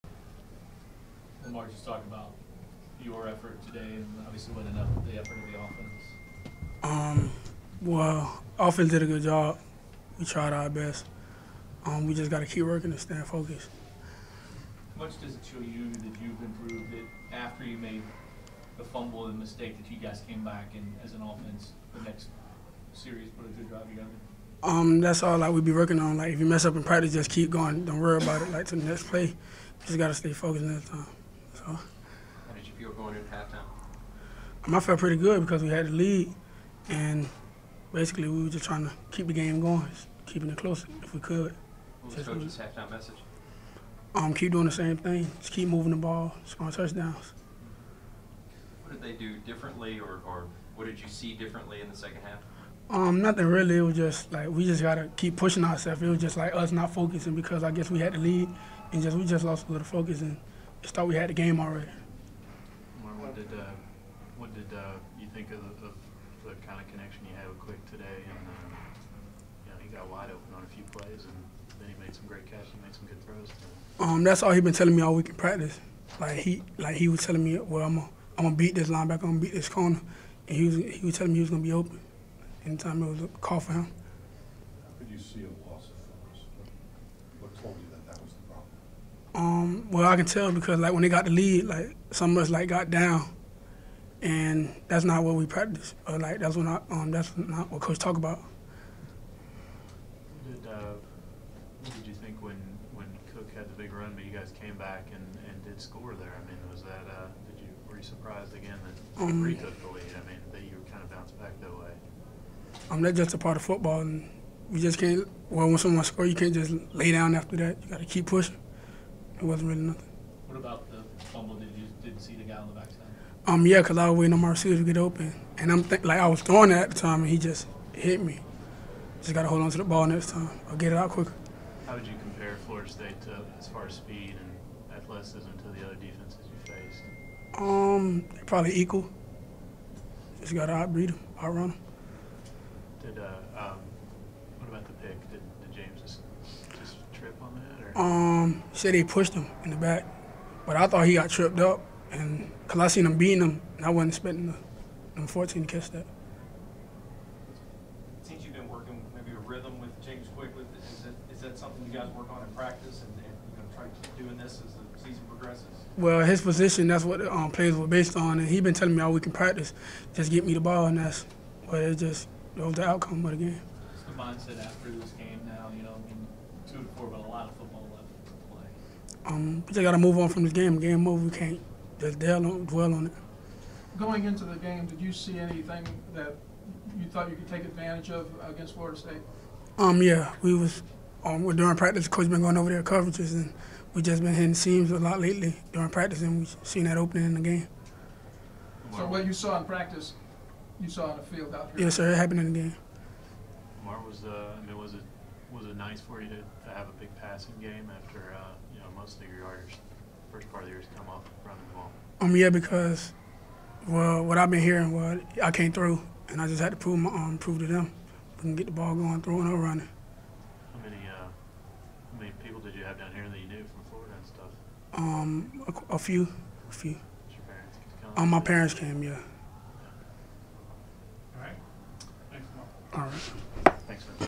Lamar Jackson Florida State Post-Game 10-17-2015
Louisville Quarterback Lamar Jackson talks after a 41-21 loss to Florida State.